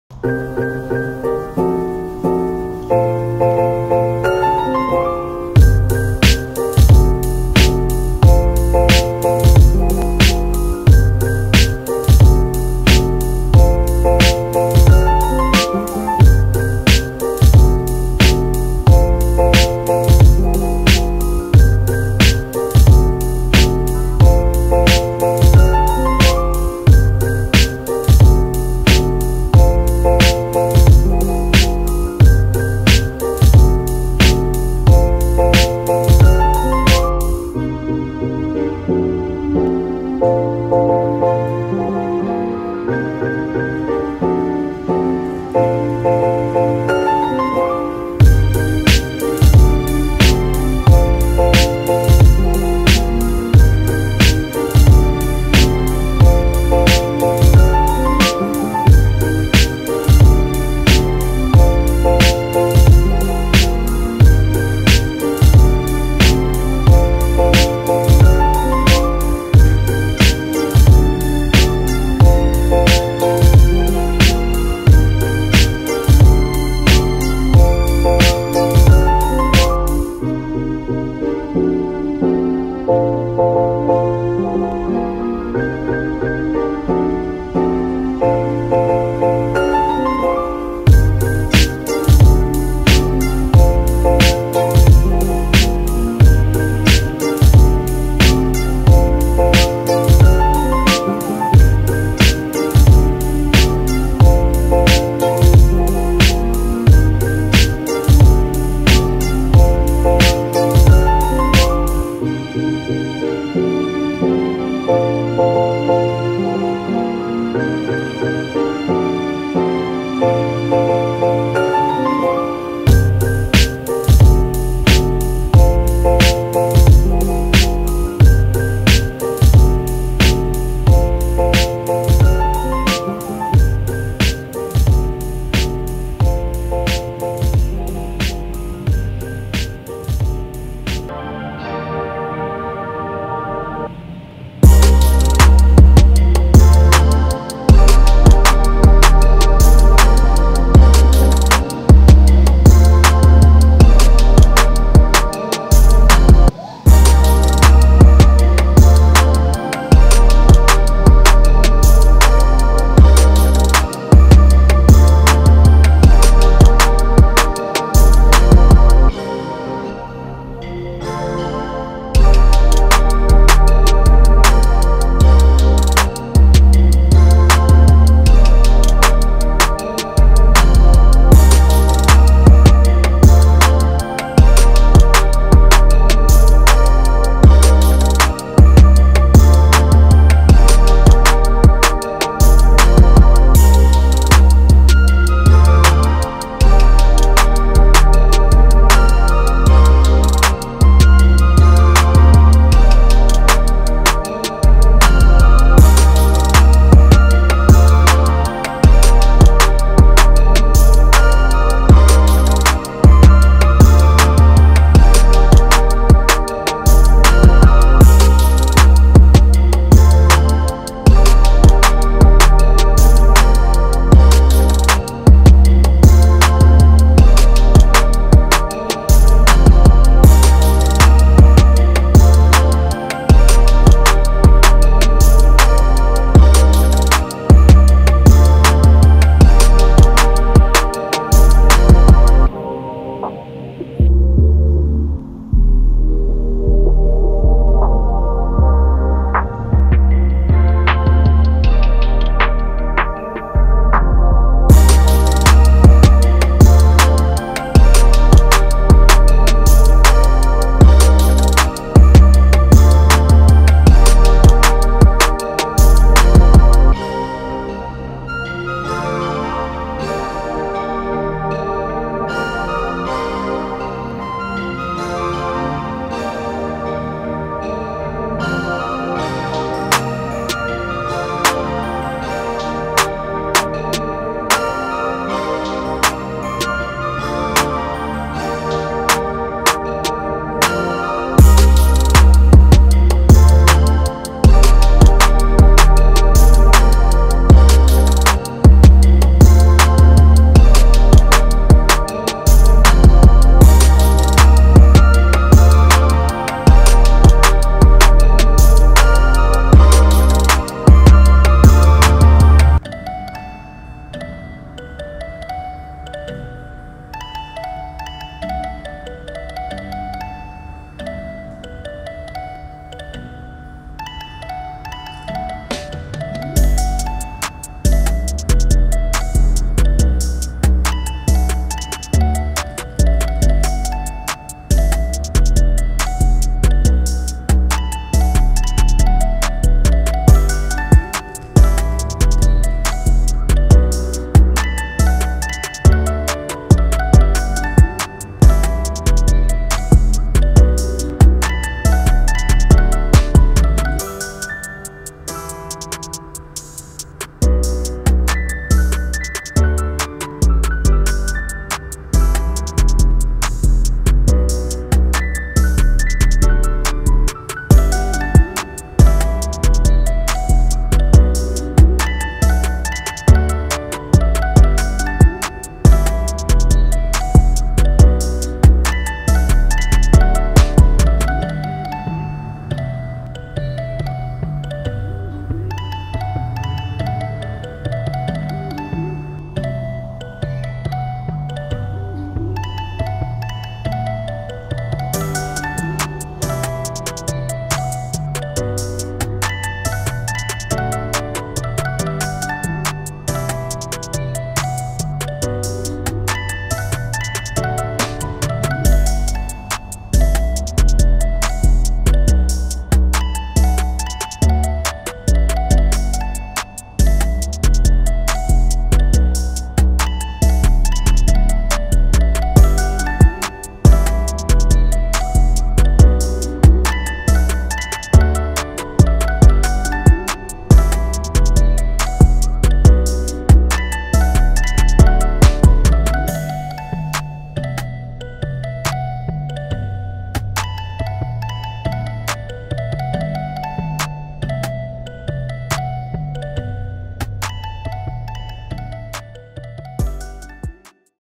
Cartoon Character TimeLapse / Cellograff #106
Spray painting another spooky ghost character using graffiti spray paint, this was done on cheap plastic wrap in an old abandoned factory.

IceCreamBuds Cartoon & Graffiti Channel